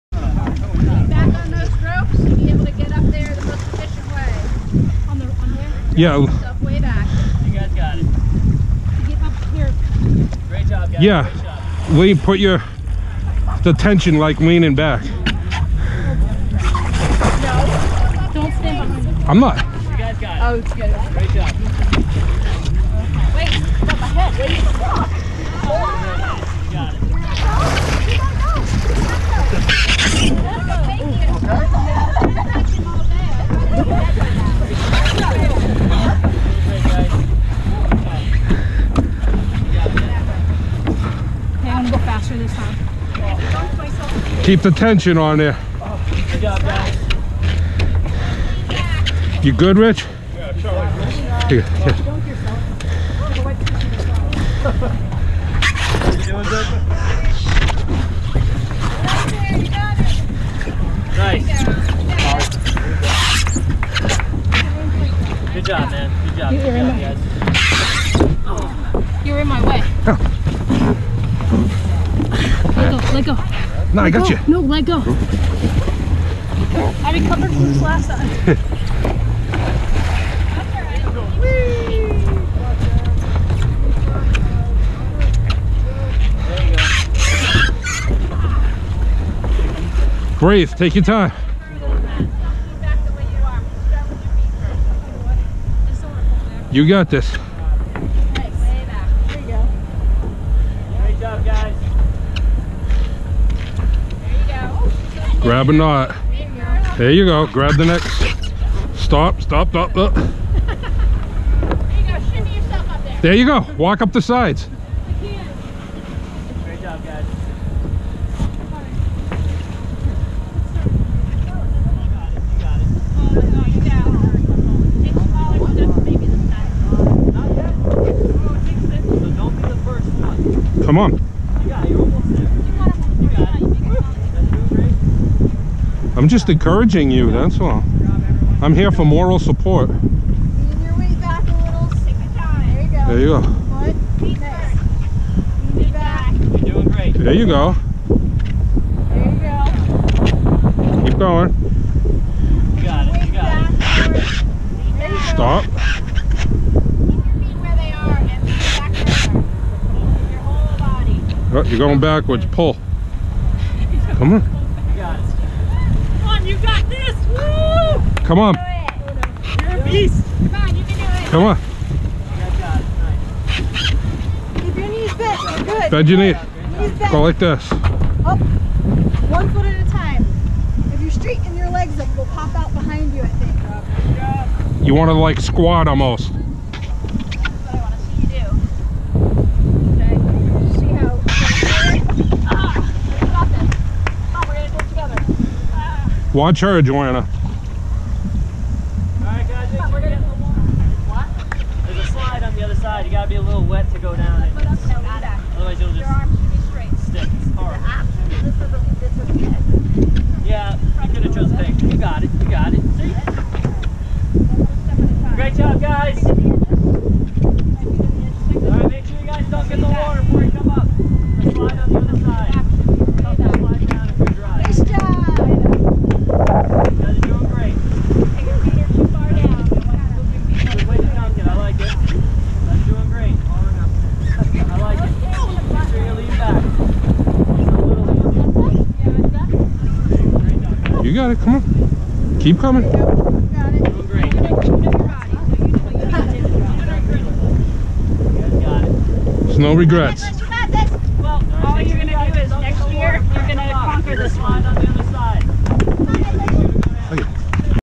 Have a good laugh on me!
THE FILTH ELEMENT OWNED ME.  I felt like the only person who couldn’t get up this slide.  I eventually called it quits and took the side up, I was in a lot of pain at that point.  Next year I will conquer this.  Have a good laugh on me.